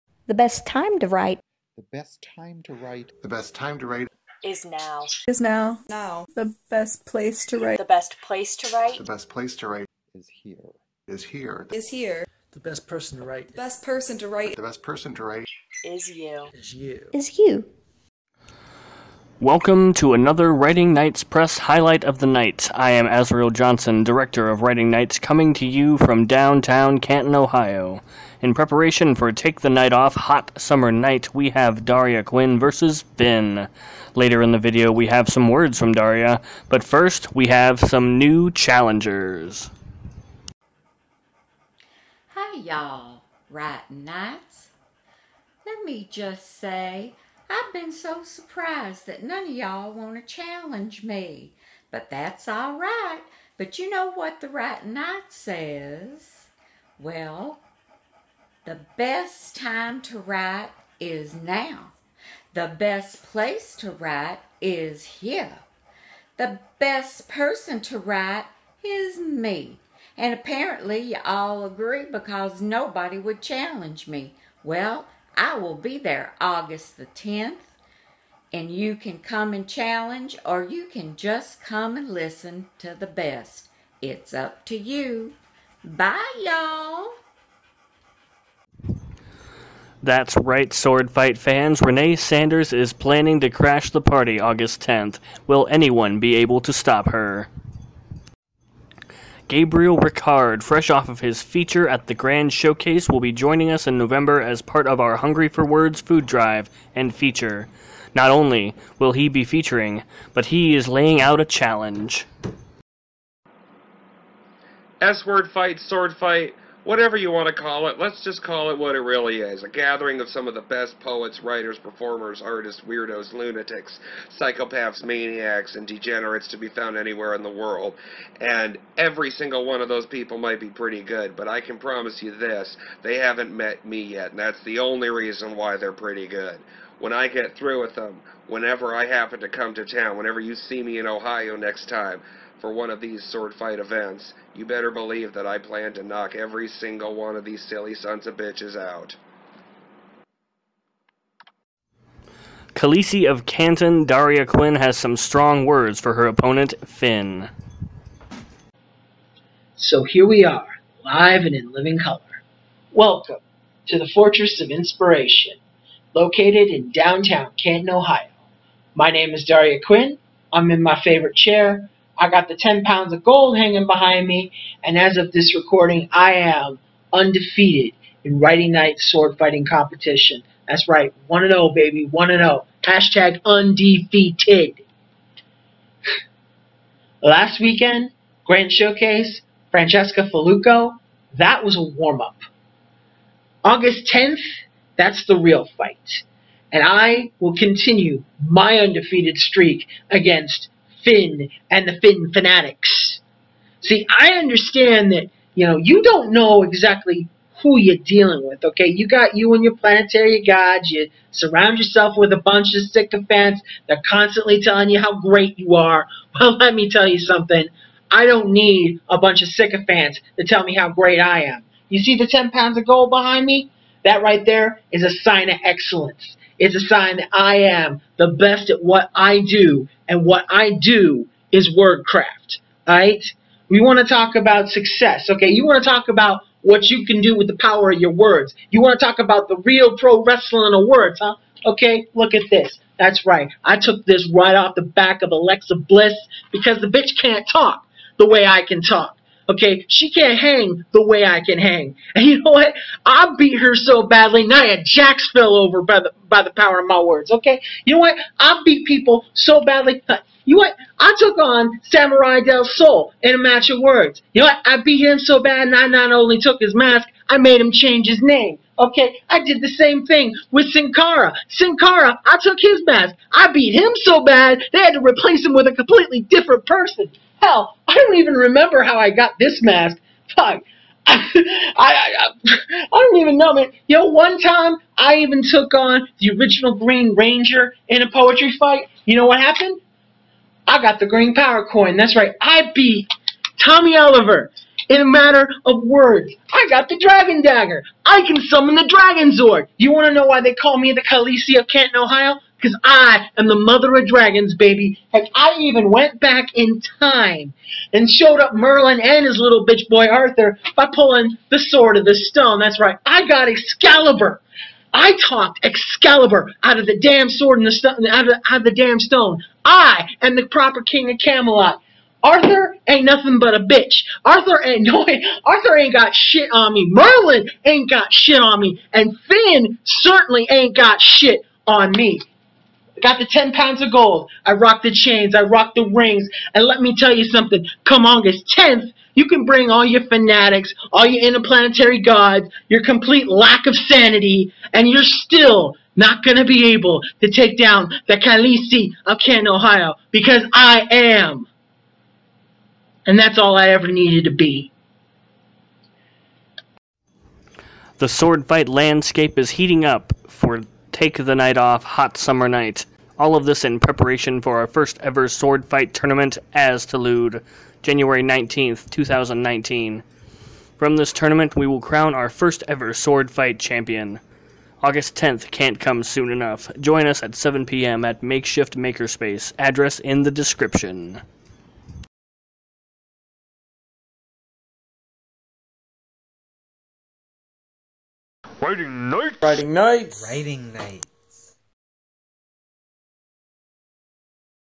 sWord Fight Promo 5 -- Highlights of the Knight (8/8/2018)
Featuring: Daria Quinn, Gabriel Ricard, Renay Sanders

Venue: Downtown Canton

Host: Azriel Johnson